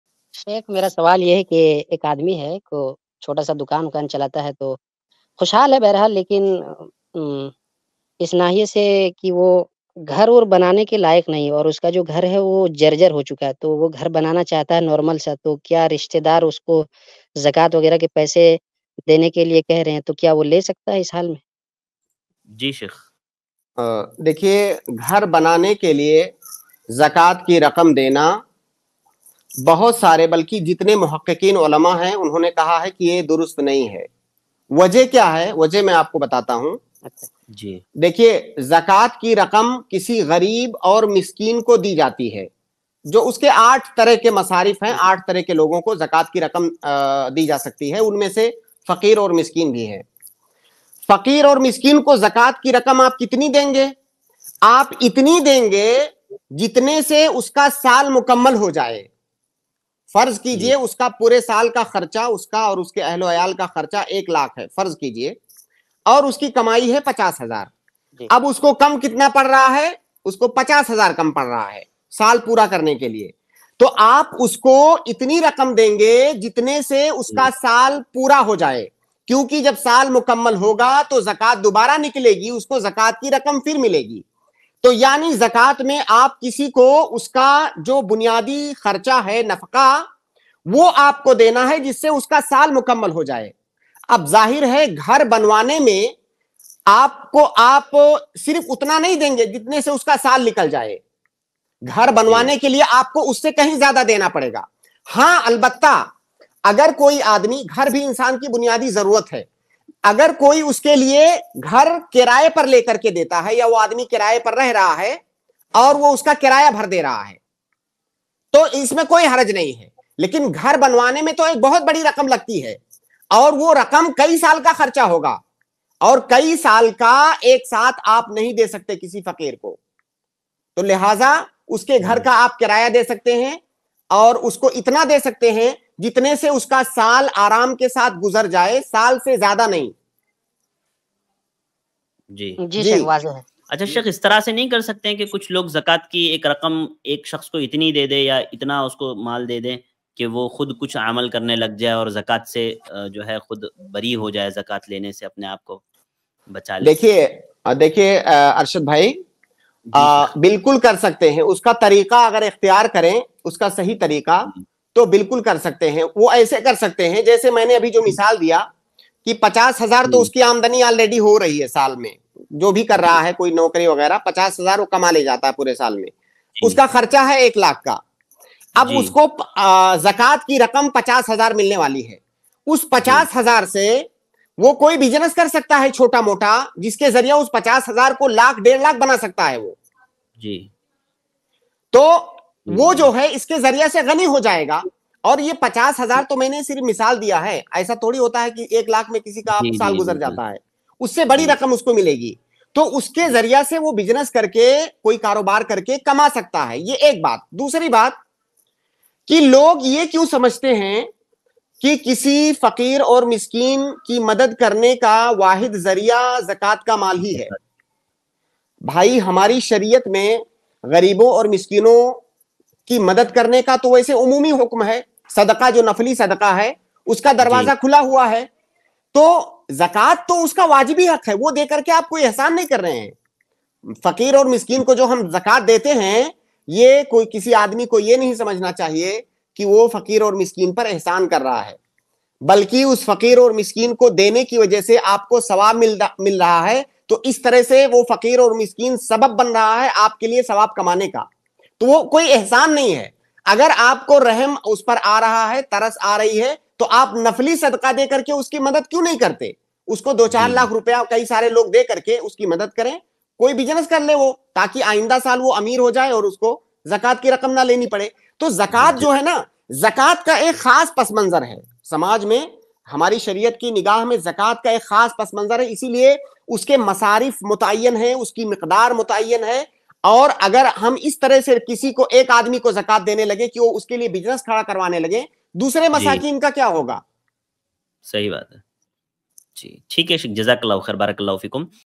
Gareeb Ko Ghar Banane Ke Liye Kya Zakat Ki Raqm De Sakte Hain? | Dr. Mohammad Naseem Madani iPlus TV
Gareeb Ko Ghar Banane Ke Liye Kya Zakat Ki Raqm De Sakte Hain? By Dr. Mohammad Naseem Madani iPlus TV Deeni Q&A

UPI : donateilm@icici